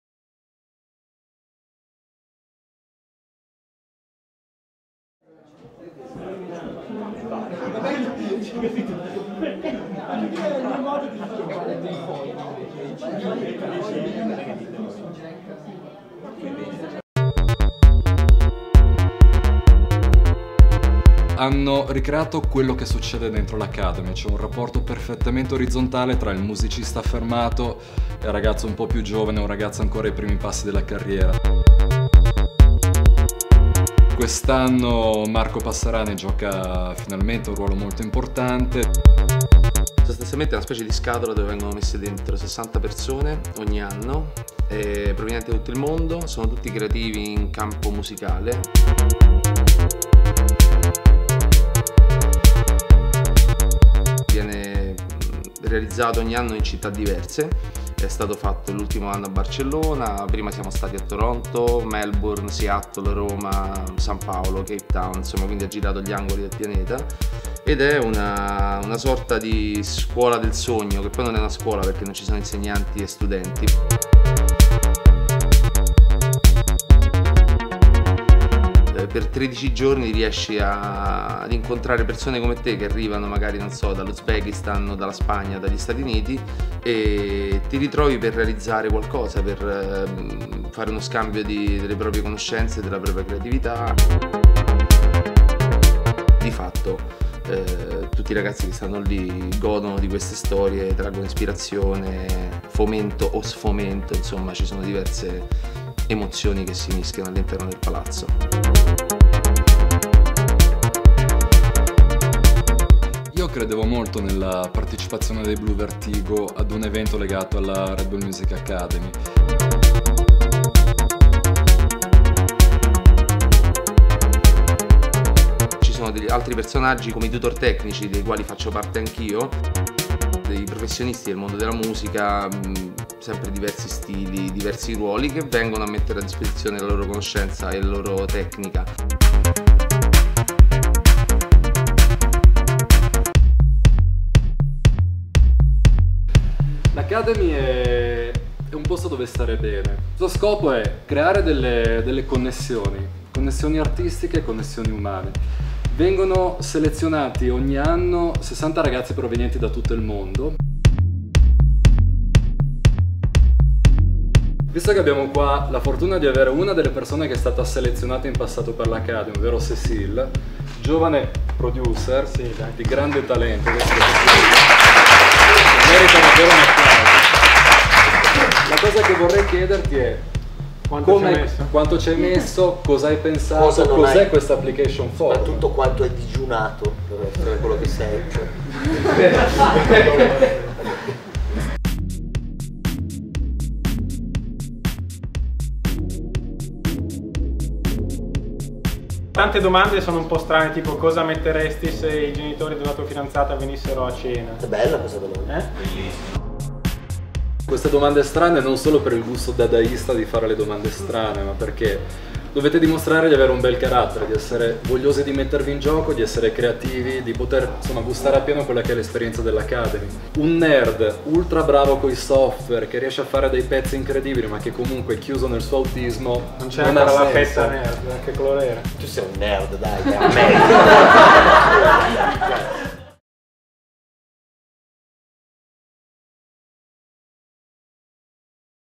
RED BULL MUSIC ACADEMY (WORKSHOP CON I BLUVERTIGO)
Ecco il primo di 4 video girati durante il Workshop che si è tenuto il 9 e il 10 giugno a Milano.
Ospiti eccelsi hanno partecipato a questa due giorni per presentare e regalare un assaggio di Music Academy ai partecipanti presenti!